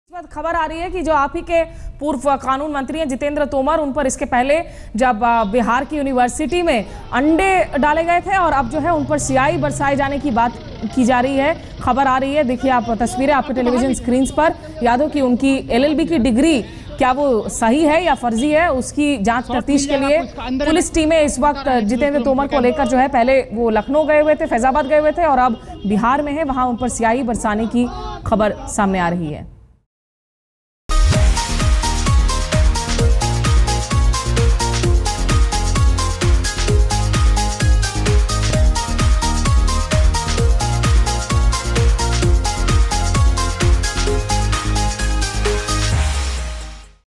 India 360: Ink Thrown On Tomar At Bihar university
Delhi law minister Jitendra Singh Tomar was greeted with rotten eggs, tomatoes and ink by members of different student organisations on his arrival at the TM Bhagalpur University.